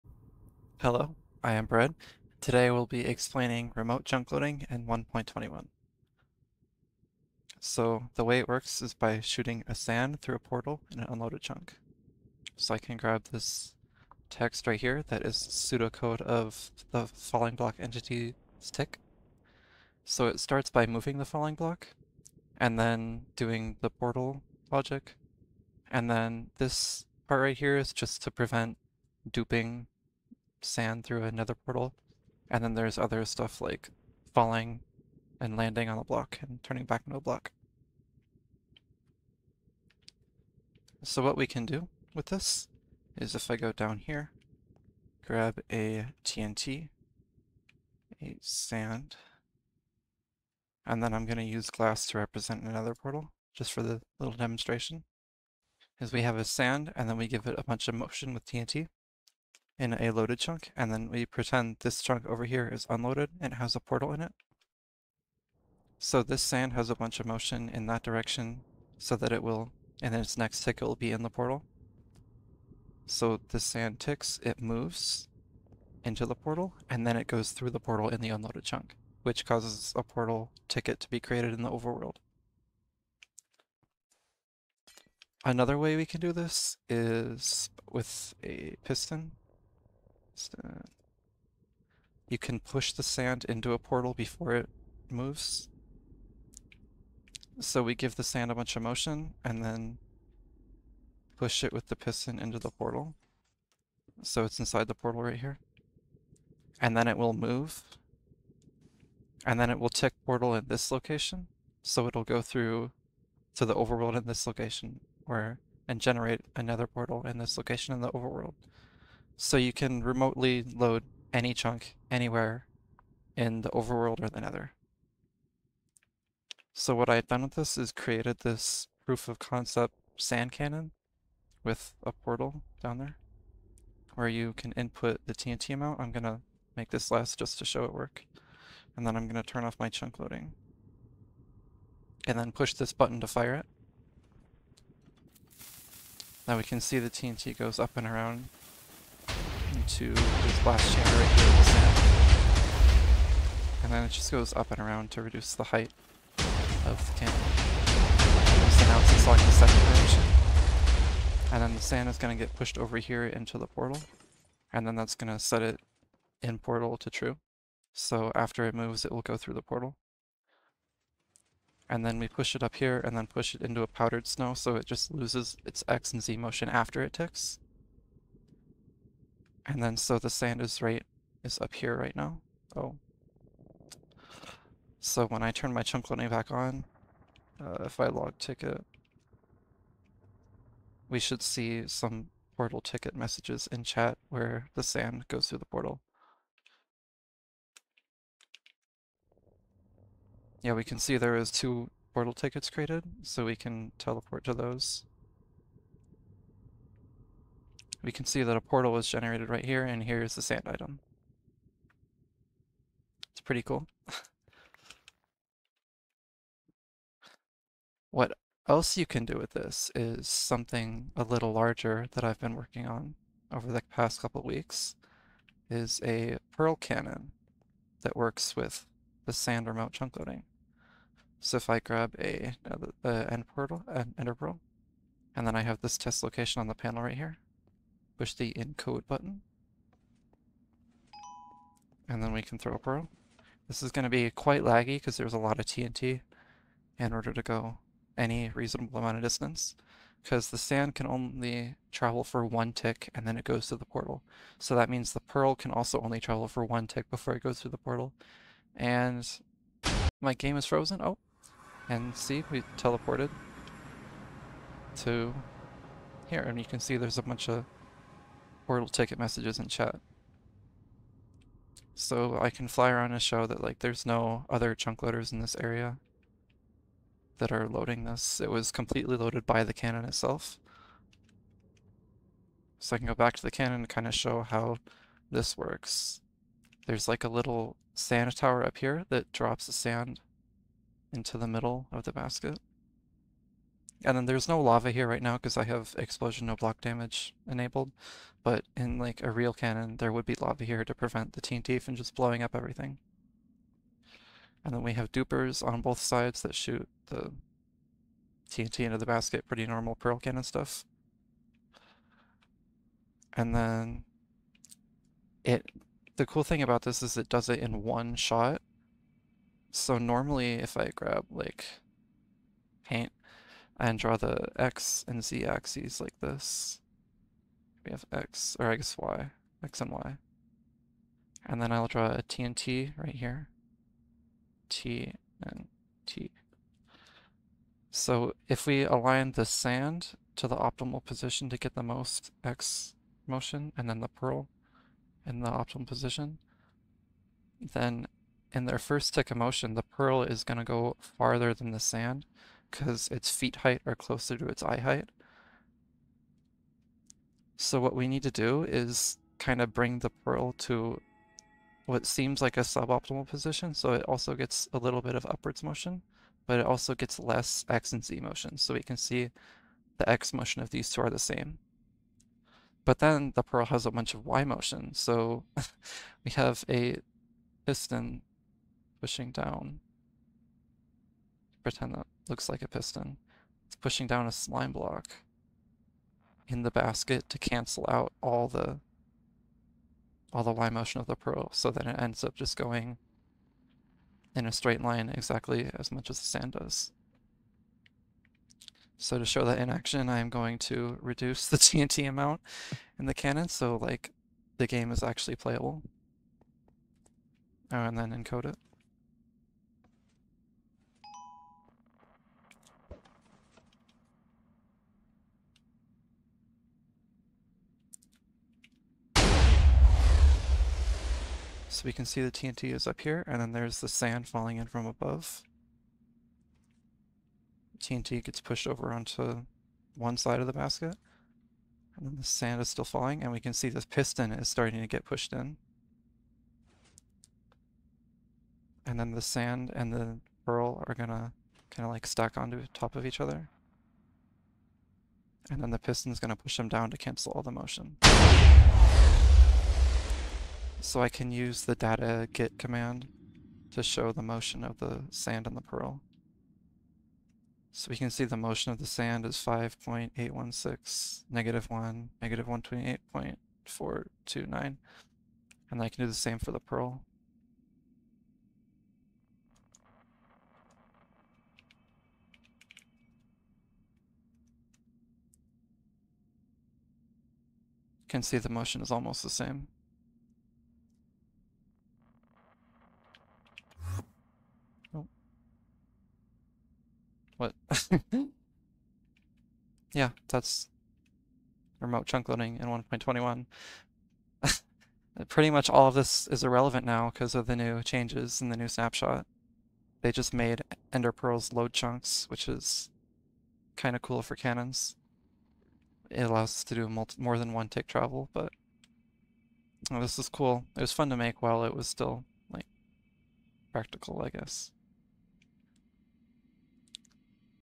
Remote chunk loading in 1.21.0 (proof of concept)
Correction: The sand doesn’t really go through the portal in an unloaded chunk. It loads the chunk as border when it moves into it, then it’s able to go through the portal.

The TNT dupers in the pearl cannon were made by TerraVoxel.
I wanted to make this video to show what I've been working on for the last few weeks before it becomes irrelevant. Both of the designs I showed off in the video are just proof of concept and not finished so I'm not going to be releasing them just yet. If you have questions about it you can ask in the chronos discord